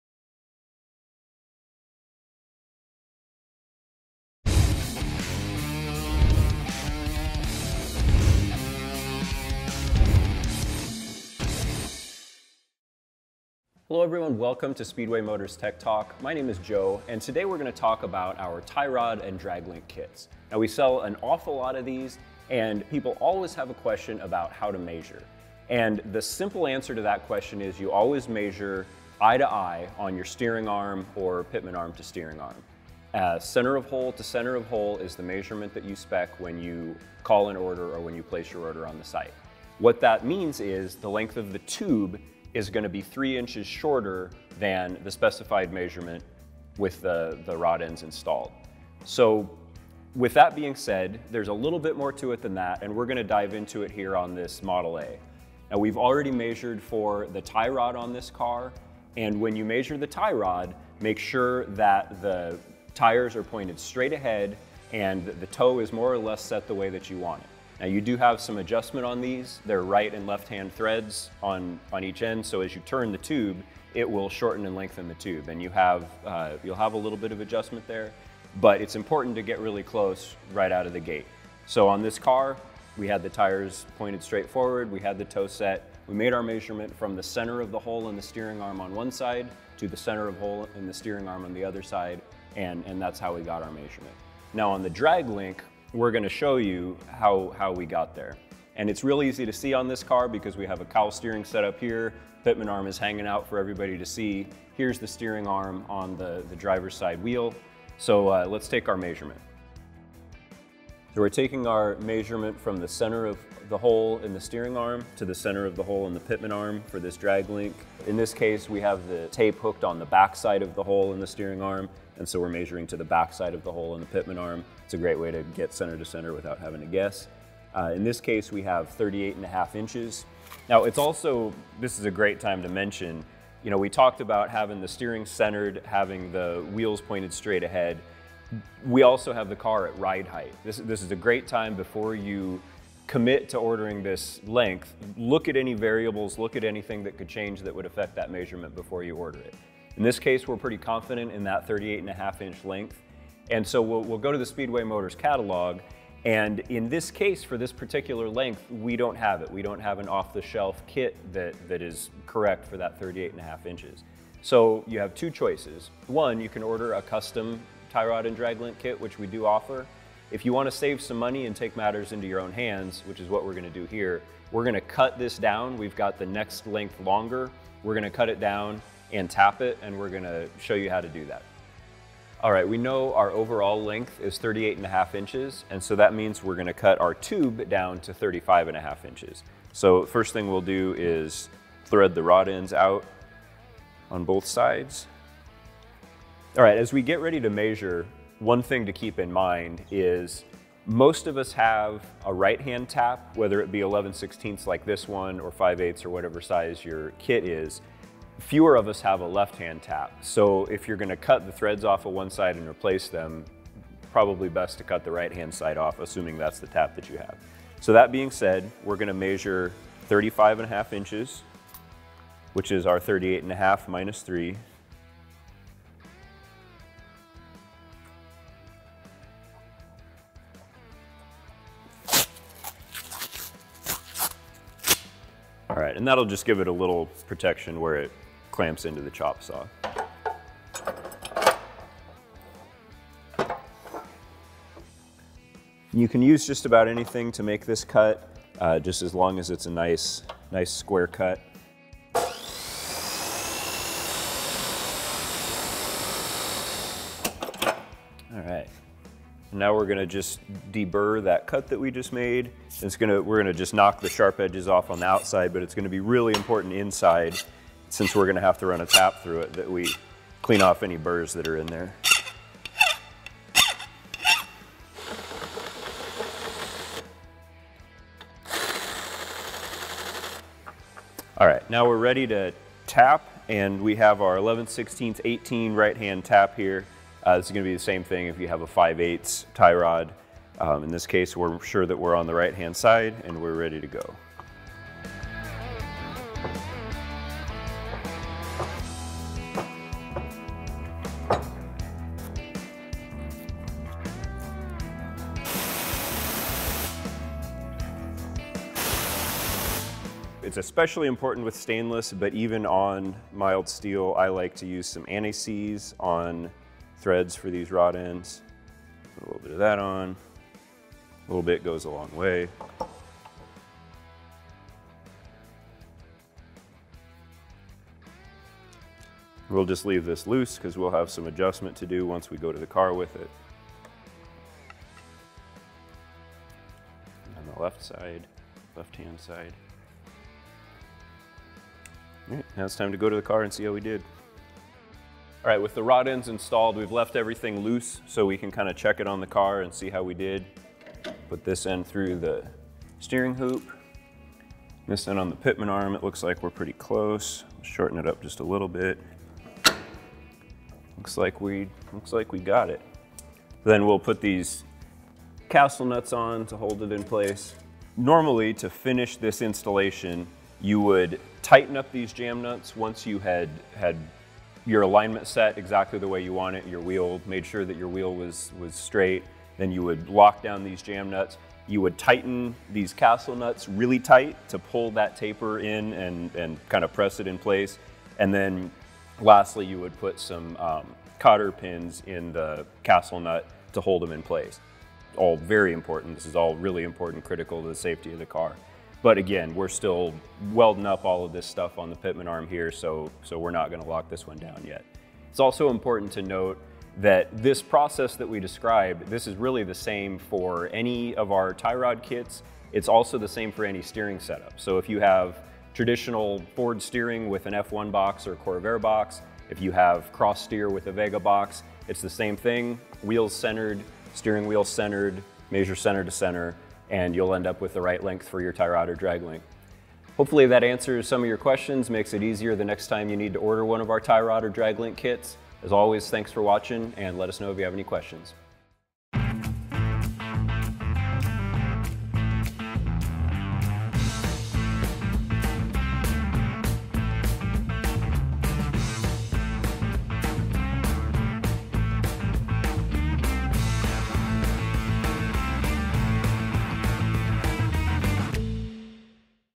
How To Fit Your Tie Rod and Drag Link
Joe McCollough explains how to measure your tie rod and drag links before you order. It is the same process when measuring any tie rod kit and steering set up. This process will work on any of our tie rods and drag links, as well as any tie rod and drag link kits. Joe demonstrates measuring on his traditional Model A build, but it will also work on anything like traditional Ford steering with an F1 box or a Corvair box, cross steer with a Vega box, and others. Joe also demonstrates how to cut and tap a drag link for rod ends if you need to cut down your drag link or tie rod to fit your build.

Tips:

Measure eye-to-eye. Use center of hole to center of hole measurement to place an order.
Keep your tape consistent. If the tape measure is on the back side of the first hole, measure to the backside of the second.
Look for anything that could change the measurement. For example, is the car on blocks? Is it at ride height? Will you change anything between now and the finished project?
When cutting and tapping, most people only have a right-hand tap. Cut the right-hand side down so you can re-tap and maintain consistent threading. If you cut the left side, you'll ruin the link's opposed threading, which allows for adjustment.
On stainless (and even mild steel), a little anti-seize goes a long way to prevent future headaches.

Products featured in this video:
Plain Tie Rod/Drag Link Kit with 11/16 Tie Rod Ends

Chrome Tie Rod/Drag Link Kit with Stainless 11/16 Inch Tie Rod Ends

Chrome Tie Rod/Drag Link Kit with 5/8 Heim Rod Ends

Plain Tie Rod/Drag Link Kit with 5/8 Heim Rod Ends

Stainless Tie Rod/Drag Link Kit with 5/8 Heim Rod Ends, Custom Length

Plain Tie Rod/Drag Link Kit with 11/16 Tie Rod Ends, Custom Length